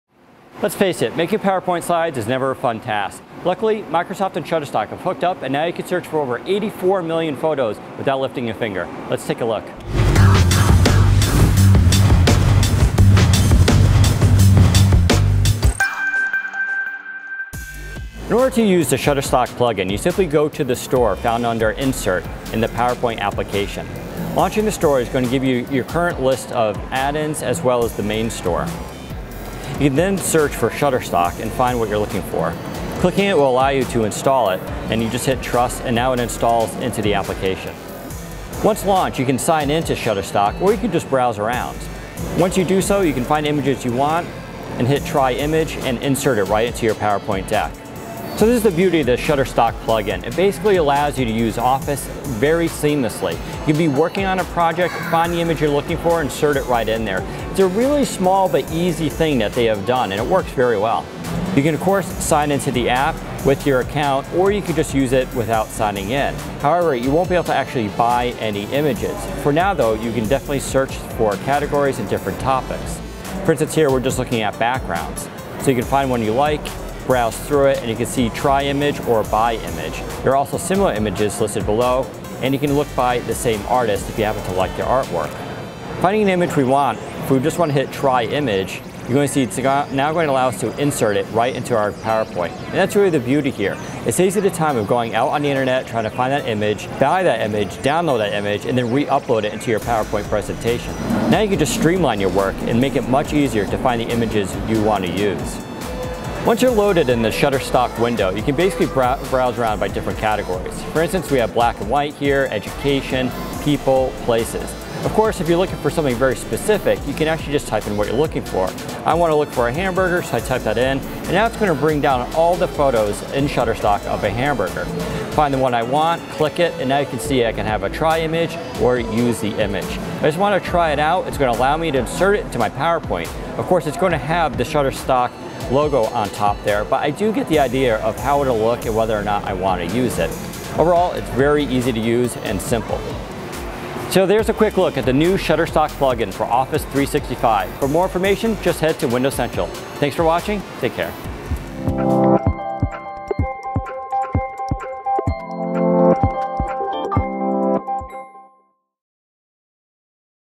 Shutterstock Will Improve Your Powerpoints
People often wonder how to insert pictures into Powerpoint without bumping into copyright or licensing issues, but the Microsoft Shutterstock plug-in for Powerpoint can help solve those issues.